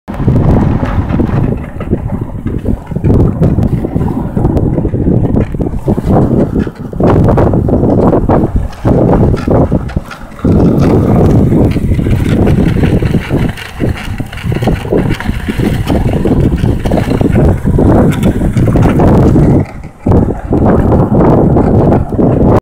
Dog sledding in the arctic
Am sure one lucky gal to experience this. One hour dog sledding up in the arctic with Danny Beck.